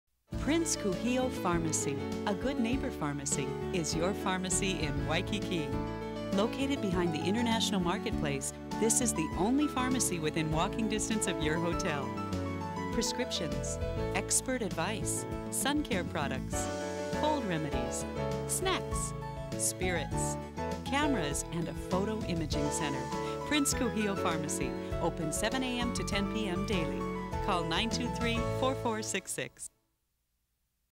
"Prince Kuhio Pharmacy" TV:30
For all of your pharmaceutical needs, Kuhio Pharmacy is your one stop shop...conveniently located in two locations in Waikiki.